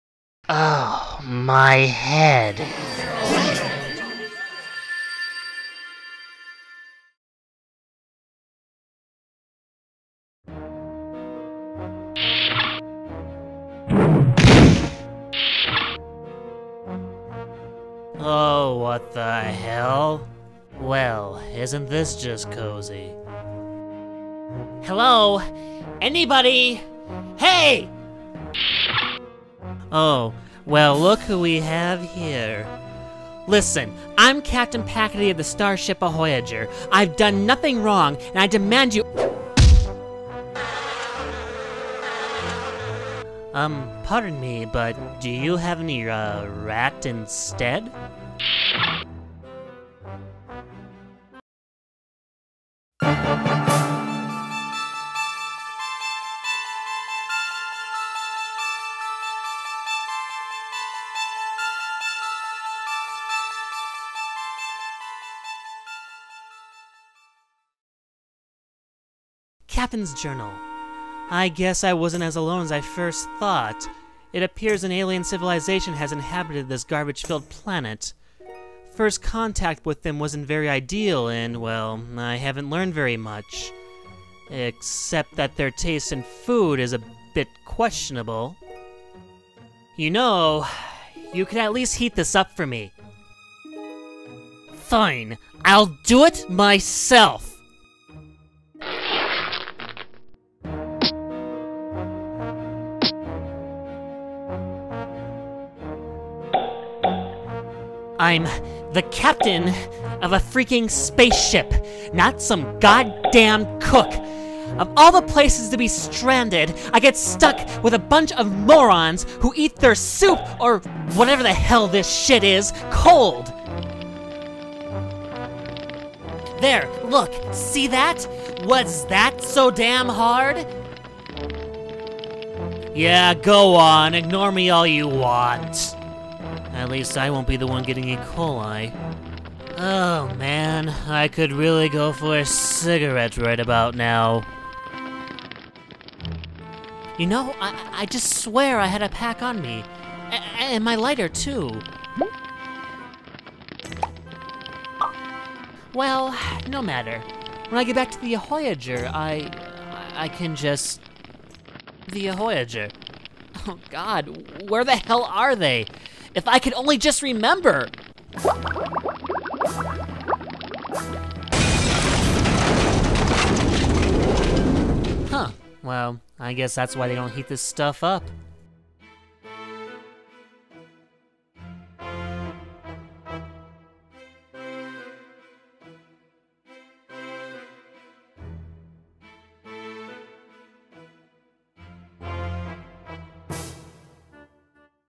I Am Ahoyager - Episode 2
Trapped in a holding cell, Pakaday is given time to think of her situation.

Film History:

Taken from the Lilredhead Studios (written July 15th, 2007):

Well, it's been almost a whole year since the series' first episode. But now the series is back on track and I'm ready to move forward with it. A plot was written a year ago for the second episode and I even started work on it. However, after some debate I decided to scrap it and take it in another direction. And that's what we have here. Enjoy!

Directed by: Chad Troftgruben
Animation by: Chad Troftgruben
Written by: Chad Troftgruben
Edited by: Chad Troftgruben
Starring: Chad Troftgruben
Released: July 15th, 2007
Runtime: 3 Minutes, 9 Seconds
Type: Animation
Genre: Comedy, Science Fiction and Fan Film